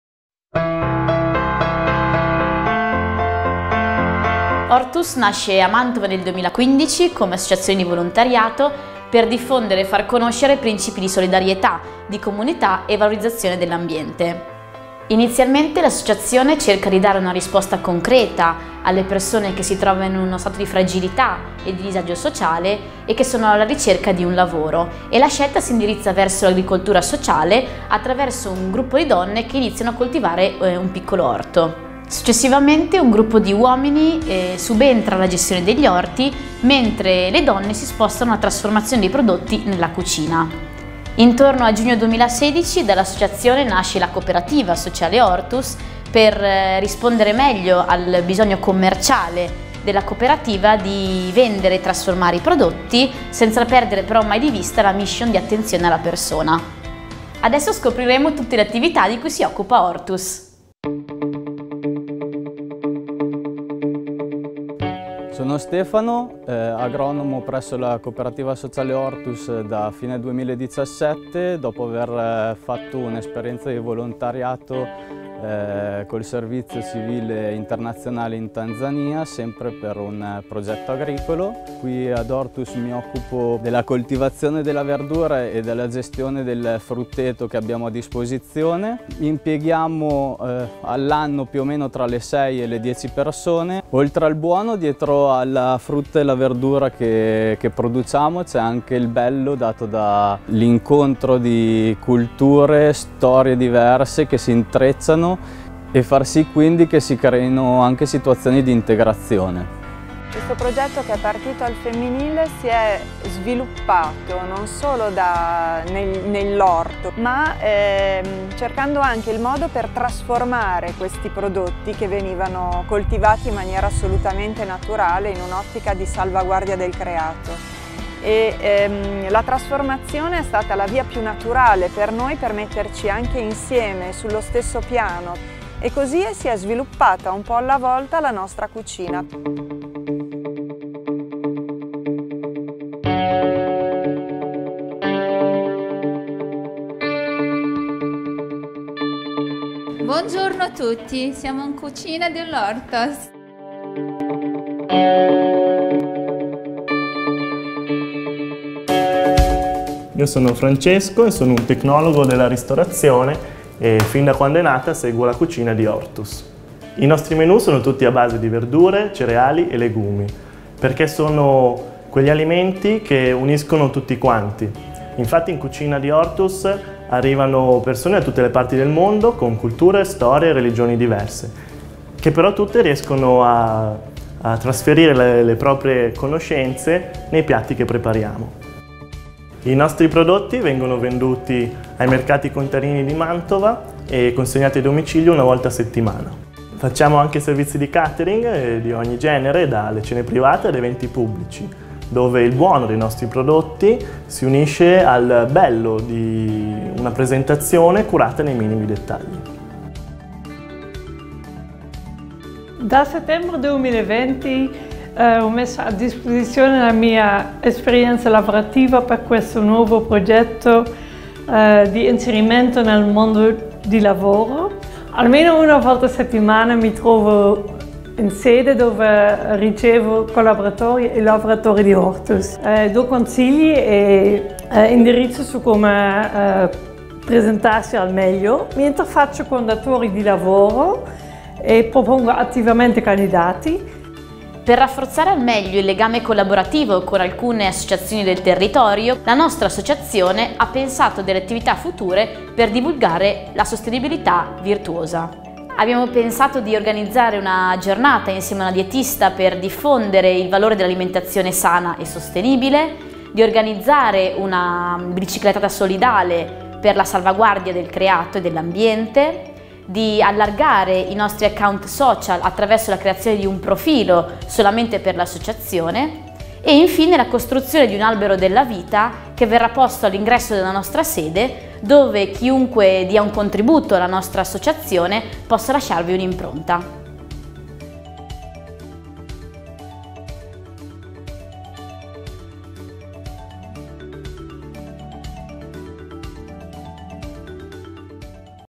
Progetto 2021 - Hortus et Labora
"“Hortus et Labora” è un progetto di Hortus OdV che si pone due obiettivi: potenziare lo sportello di orientamento al lavoro già attivo da settembre 2020 e programmare eventi e modalità di promozione mirati.
Lo scopo primo è quello di diffondere sempre di più il concetto di carità proattiva, ovvero inquadrare le persone che vivono momenti di fragilità o di difficoltà, non unicamente come persone nel bisogno, ma anche persone con capacità e risorse da mettere a frutto. Questo quindi si traduce in un accompagnamento nella ricerca del lavoro, con la cura e le attenzioni di un servizio diocesano, ma anche con una professionalità specifica e una finalità rivolta all’autonomia della persona.
Persone con storie difficili alle spalle e spesso inoccupate da tempo, trovano a Hortus una possibilità di ripartenza con l’attivazione di tirocini di inclusione sociale. Tramite lavori negli orti o in cucina riprendono dimestichezza con le dinamiche lavorative (responsabilità, orari, lavoro in gruppo, relazioni positive, obiettivi, risultati). Parallelamente vengono invitate a dei colloqui specifici per preparare documenti e curriculum, focalizzare interessi e possibilità, motivare nella ricerca autonoma del lavoro e allo stesso tempo organizzare una rete di contatti per eventuali inserimenti lavorativi nelle aziende del territorio.

Il secondo obiettivo del nostro progetto è anche diffondere ed educare la cittadinanza a questa lettura per certi versi nuova e rivoluzionaria. L’ecologia integrale non può prescindere dall’impegno su temi sociali, ambientali, e di educazione alimentare.
“Hortus et Labora” prevede per questo:
- un percorso con la dietista Valentina Muollo, pensato per promuovere un’alimentazione sana e attenta al pianeta, che prevede approfondimenti online e un incontro finale dal vivo.
- una biciclettata presso la Foresta della Carpaneta (sito di ERSAF) per una giornata di festa e di comunità a impatto zero
- la creazione e la diffusione di account social (facebook e instagram) di Hortus OdV per raggiungere nel tempo e con continuità diverse tipologie di persone, modulando i nostri valori con le modalità più adeguate
- l’inaugurazione di un “albero della vita” da posizionare nella nostra sede e sul quale chiunque collabori con noi, potrà lasciare fisicamente la propria impronta. Questo ancora una volta per sottolineare l’importanza delle relazioni e della differenza che ogni gesto, seppur piccolo ed unico, concorre a costruire qualcosa di più grande in cui riconoscersi tutti e tutte.

Da sempre ci siamo mossi ispirandoci all’enciclica Laudato si’, dove “Tutto è connesso” si traduce in “Tutto è in relazione, e che la cura autentica della nostra stessa vita e delle nostre relazioni con la natura è inseparabile dalla fraternità, dalla giustizia e dalla fedeltà nei confronti degli altri” (n.70). Ecco quindi perché le coltivazioni di Hortus sono completamente naturali e perché rivolgiamo un’attenzione particolare ai materiali di imballaggio utilizzati nei mercati o nei catering. Anche il menu della nostra cucina è pensato per unire e non per dividere, per poter invitare tutti alla tavola imbandita: ortaggi, frutta, cereali, legumi uniti a latticini e uova, sono bagaglio culturale di ogni persona, indipendentemente dal paese d’origine. L’obiettivo di creare buone relazioni tra le persone e il Creato, anche tramite un consumo alimentare ragionato, sappiamo non può scindersi dalle buone relazioni all’interno della comunità.

Con rinnovato entusiasmo, ed una equipe formata e giovane, puntiamo sempre più a diffondere il nostro impegno, avendo ben chiari i valori costitutivi della nostra organizzazione: la centralità delle persone, la necessità di una maggiore cura del Creato, l'importanza del lavoro anche come spinta ad una inclusione effettiva nella comunità.